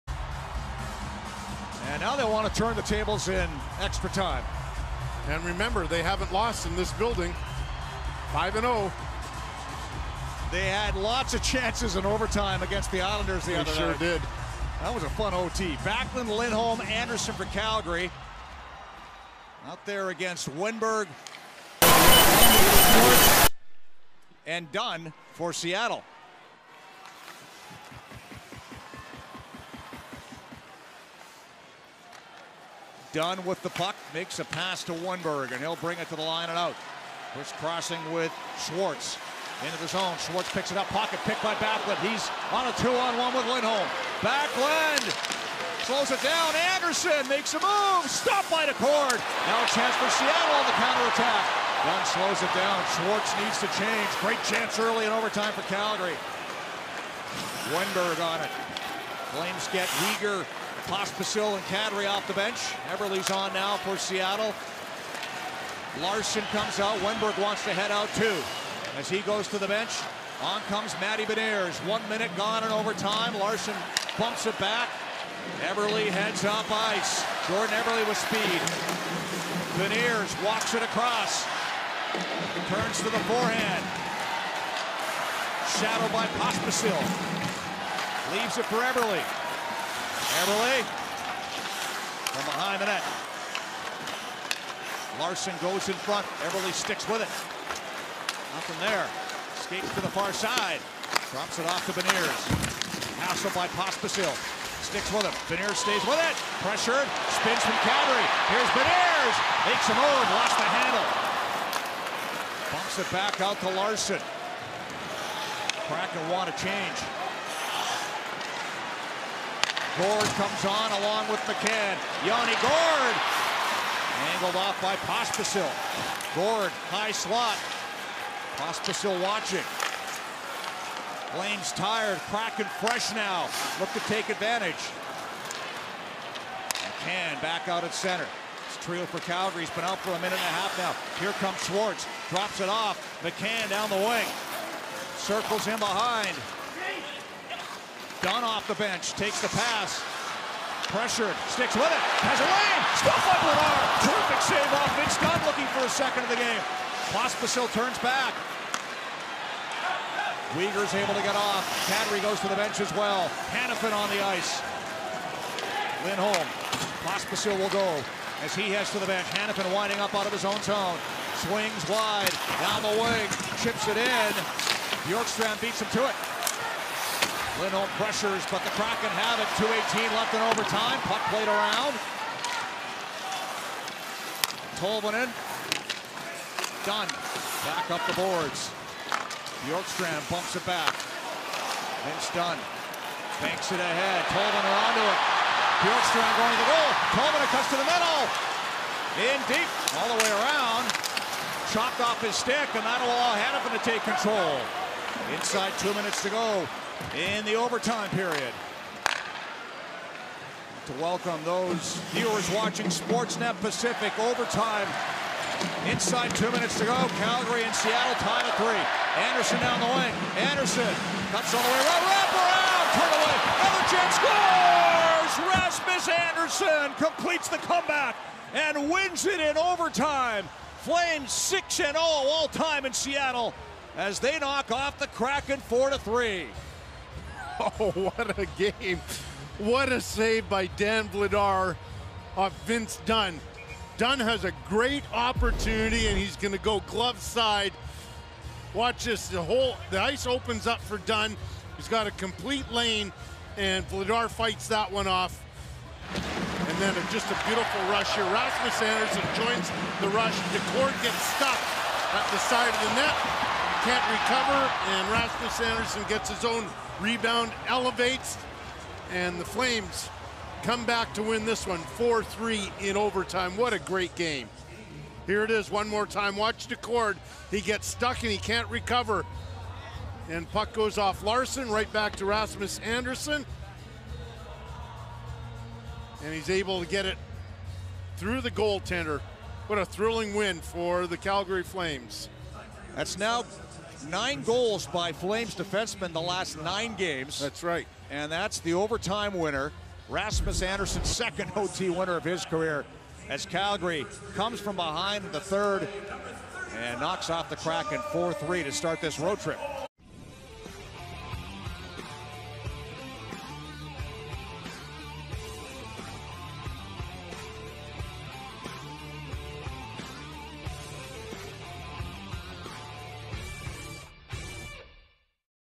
Calgary Flames at Seattle Kraken | FULL Overtime Highlights - November 20, 2023
Watch the full overtime between the Calgary Flames and Seattle Kraken from Monday, November 20, 2023.